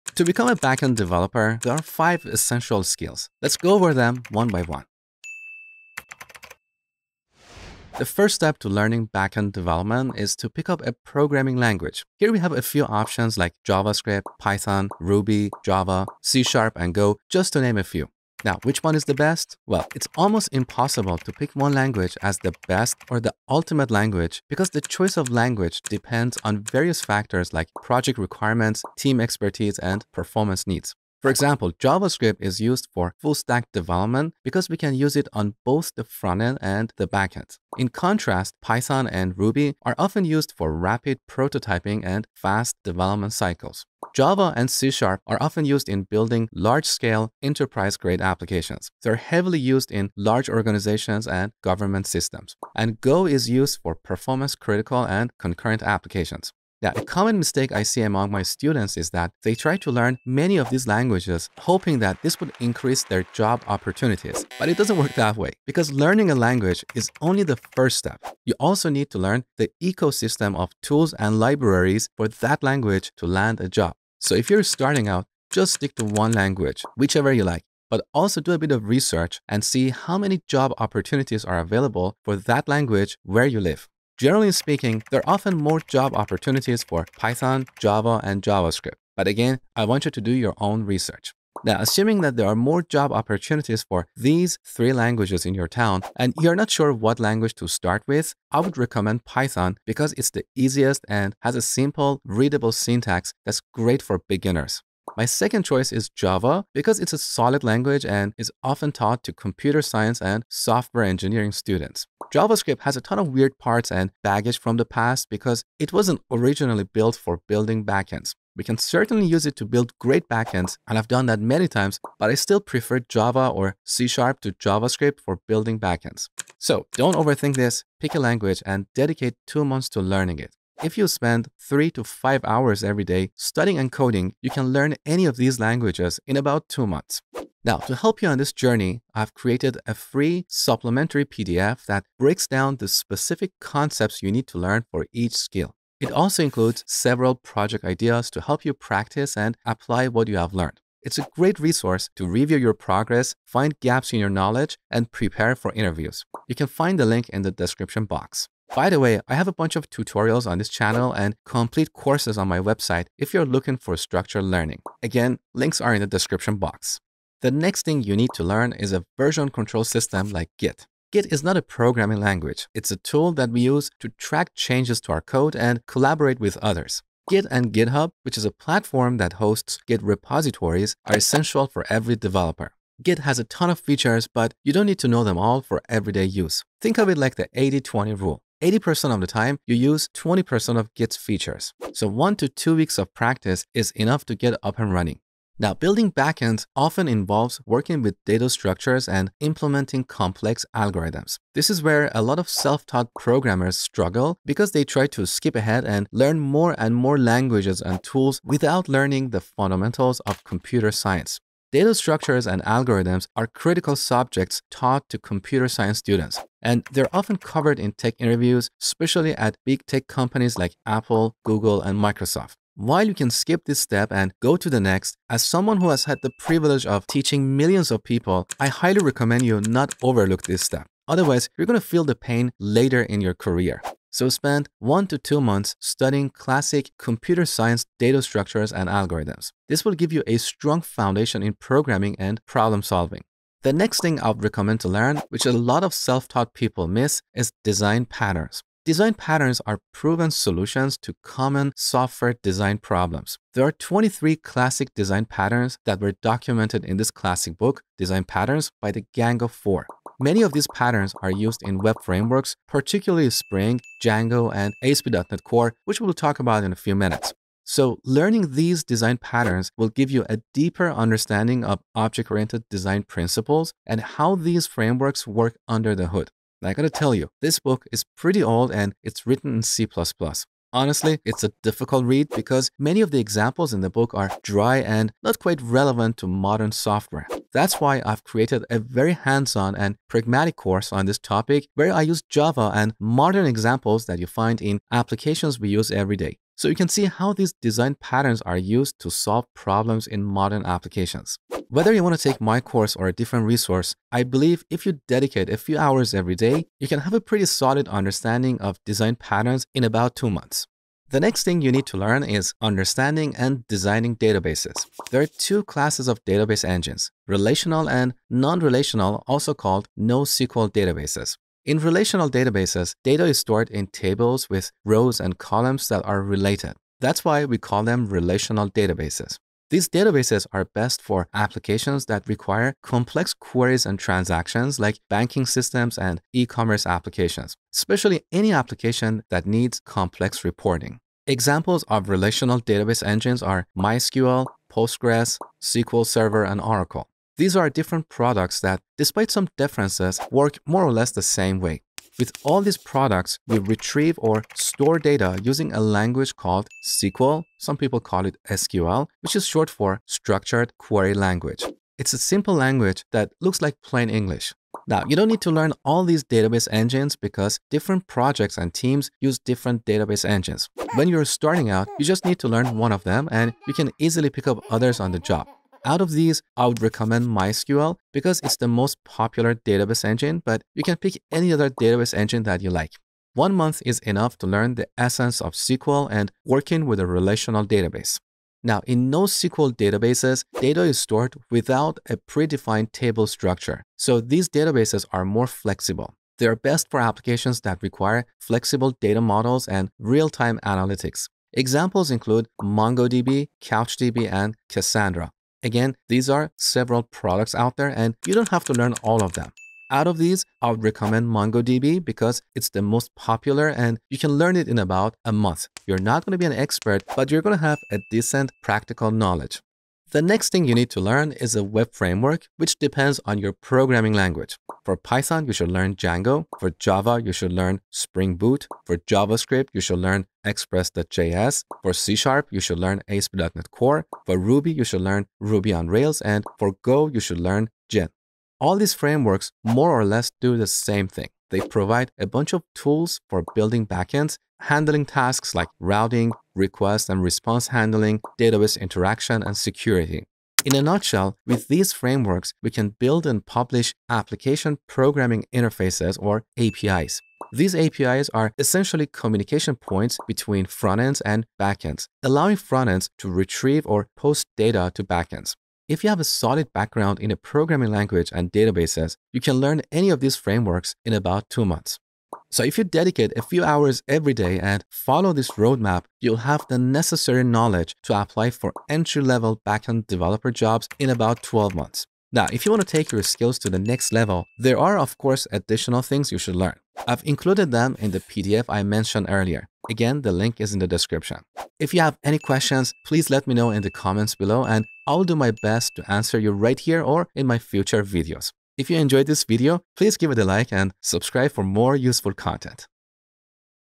The Complete Backend Developer Roadmap
Go from zero to a backend developer in 12 months. This step-by-step roadmap covers the essential skills and latest technologies to become a backend developer in 2024.

🔗 Other roadmaps

📚 Tutorials

📖 Chapters

00:00 - Introduction
00:08 - Programming languages
02:48 - Version control (Git)
03:16 - Data structures & algorithms
04:03 - Design patterns
05:10 - Databases
07:00 - Web frameworks
08:02 - Additional skills